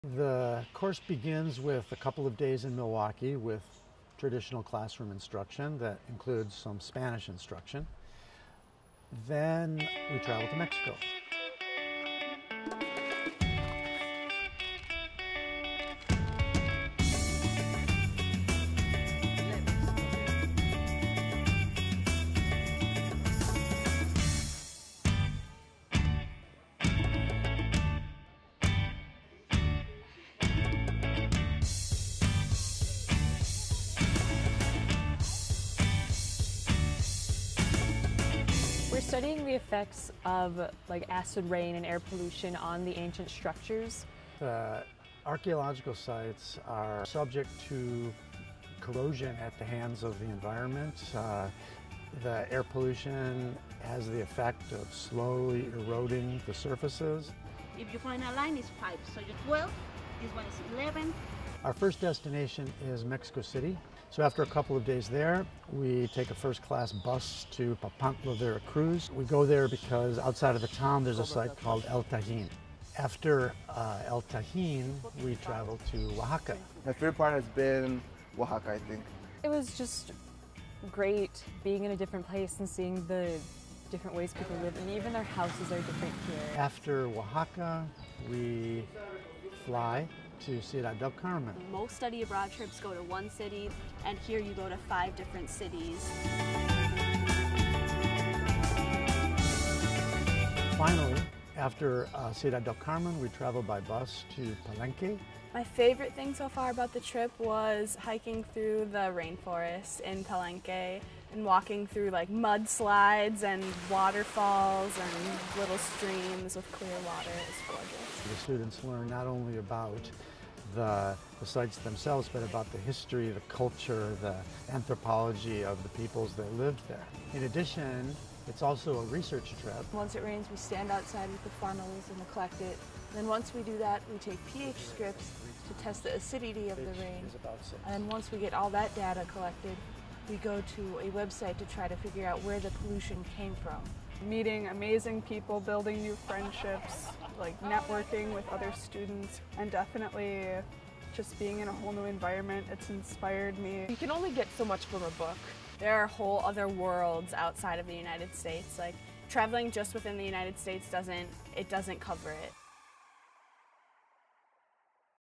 Learning amid the ruins
Follow these UWM travelers as they study the effects of acid rain on Latin American Heritage in Southern Mexico.